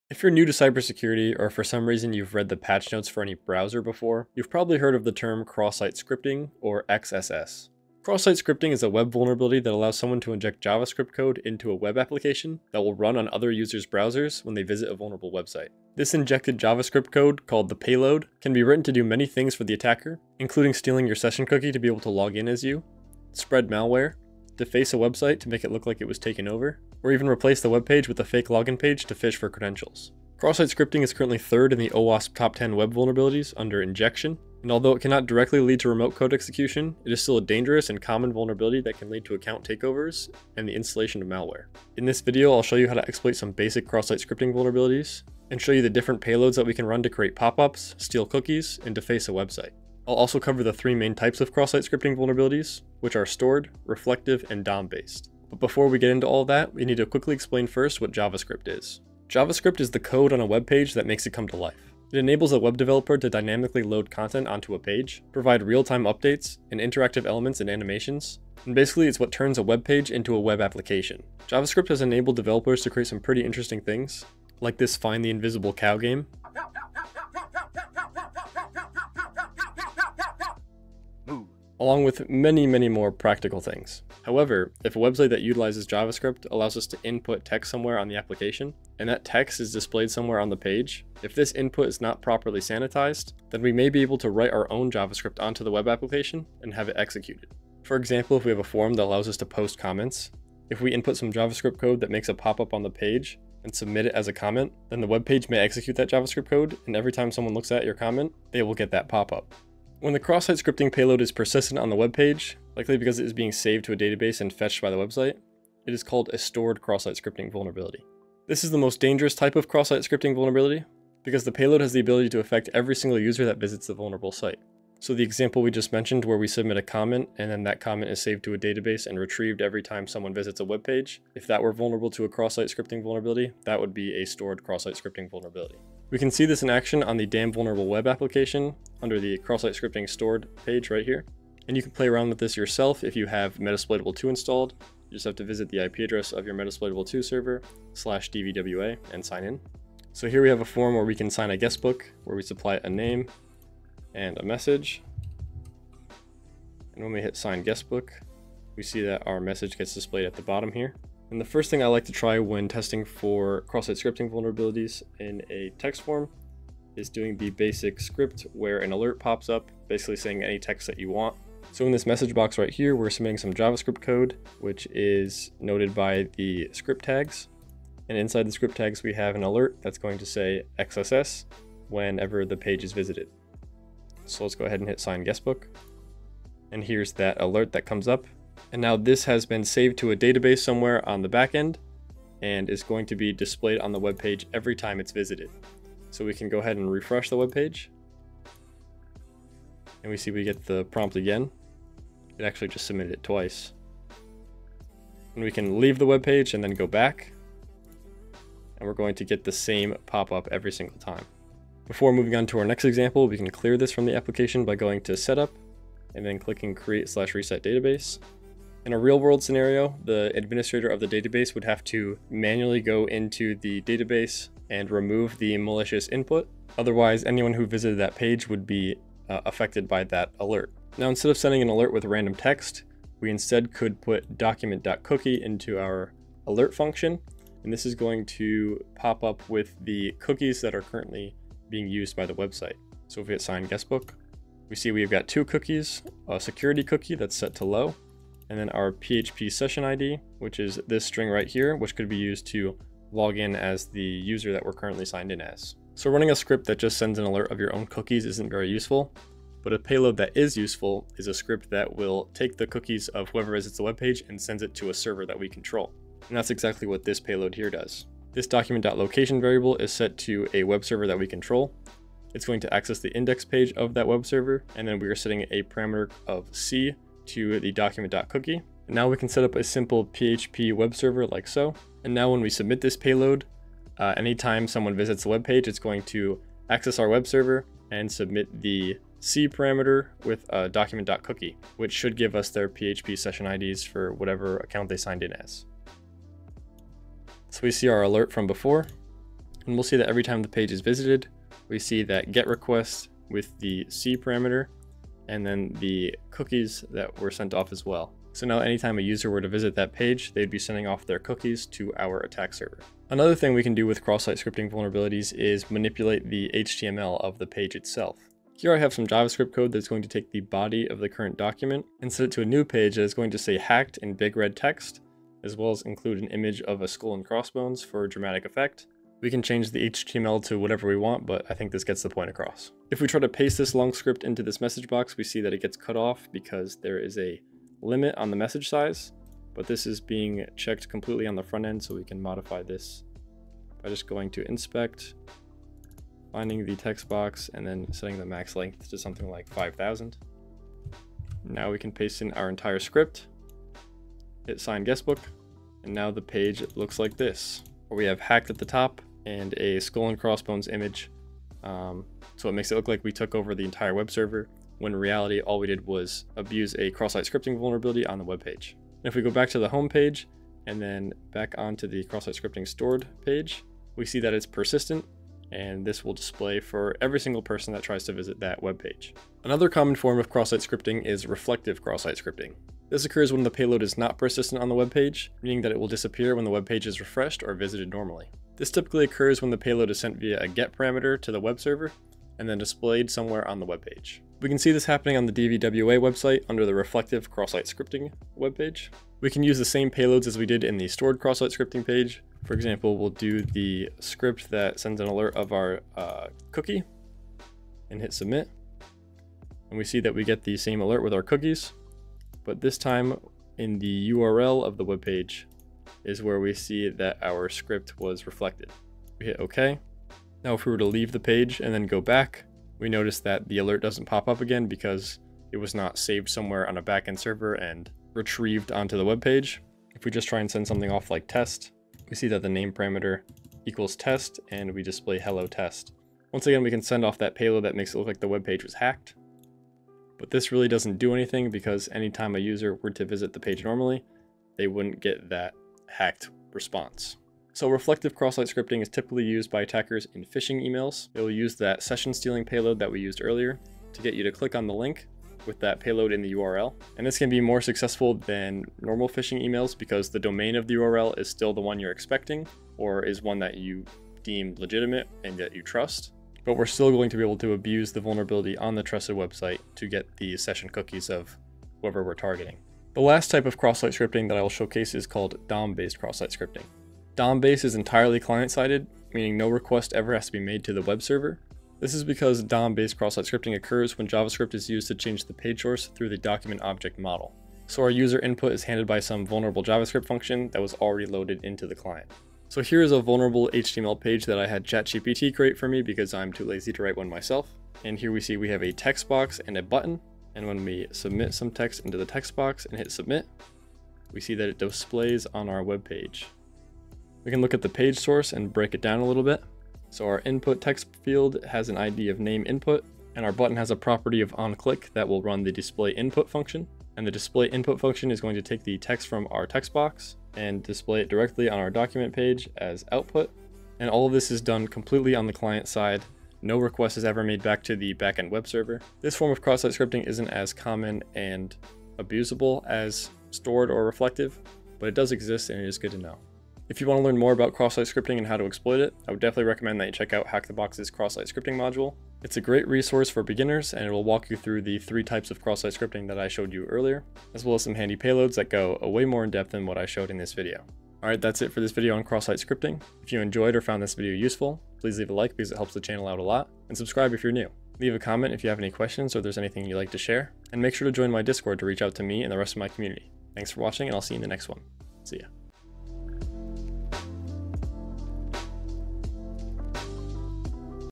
Cross Site Scripting Explained with Real Examples (Stored, Reflected, DOM XSS)
In this video, we dive deep into one of the most common web vulnerabilities: Cross-Site Scripting (XSS). If you're learning ethical hacking, penetration testing, or preparing for bug bounty hunting, this XSS tutorial is a must-watch.

I’ll show you how Cross-Site Scripting works, step-by-step, and demonstrate real examples of Stored XSS, Reflected XSS, and DOM-Based XSS on my own test site. Whether you're a cybersecurity beginner, a student studying web security, or a tech enthusiast, you’ll gain practical skills you can use to better understand and prevent client-side attacks.

You'll learn:

What XSS is and why it's dangerous

How attackers inject JavaScript into vulnerable web apps

Live XSS payload examples

How DOM-based XSS differs from reflected and stored

If you're getting into web application security, cybersecurity careers, or just want to know how hackers think, this is the perfect video for you.